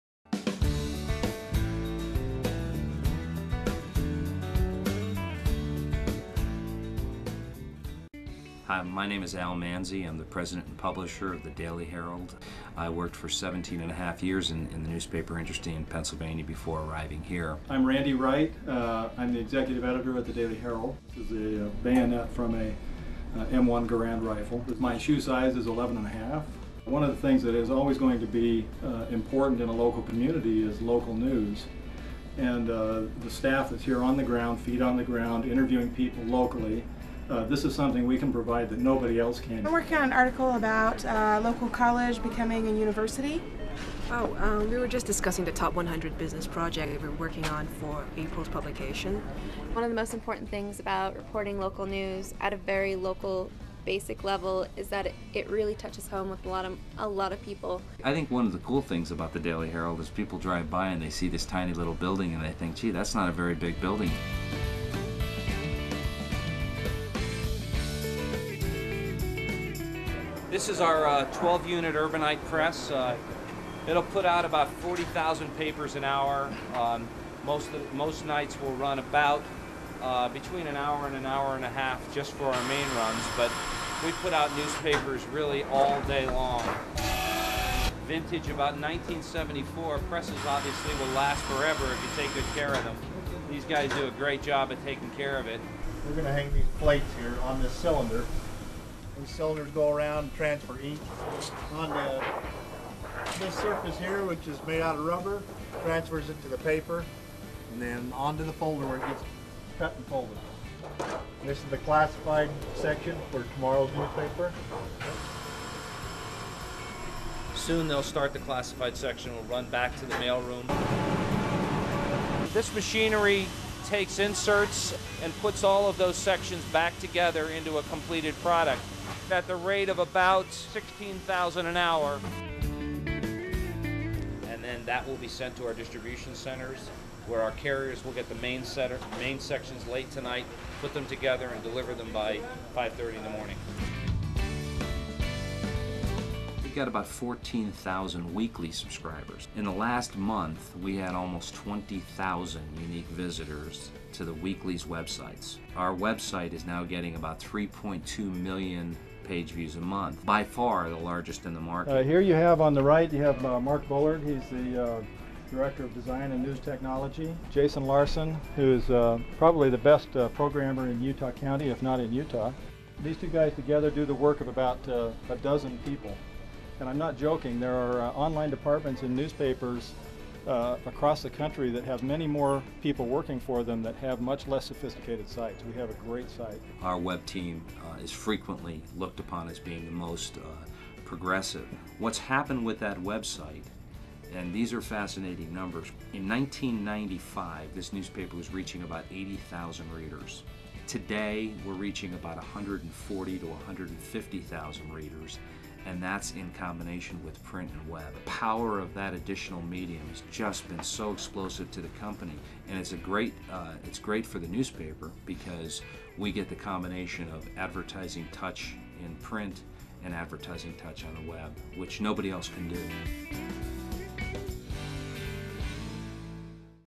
Daily Herald
We talk to the publisher, editor, and other staff members of the Daily Herald in Provo, Utah to find out how local newspapers are adapting to the ever changing media market.